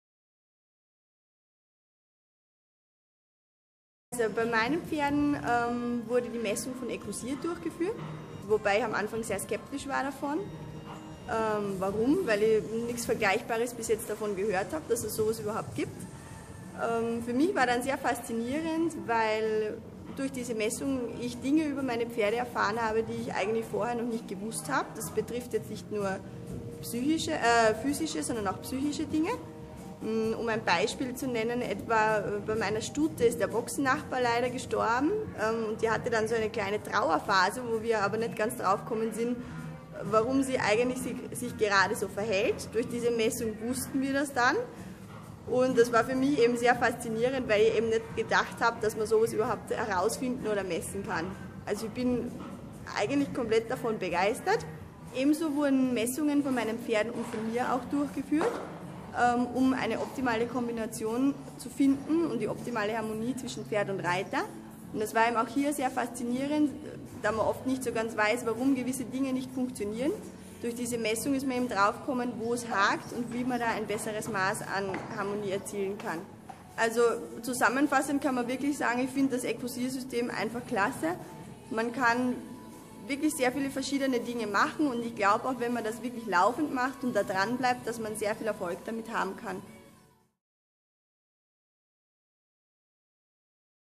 Sabrina Reinwald: "Ich habe Dinge über meine Pferde erfahren, die ich vorher nicht wusste!"
Die Dressur-Reiterin Sabrina Reinwald berichtet über ihre Erfahrungen mit der EQUUSIR BEST-BOX.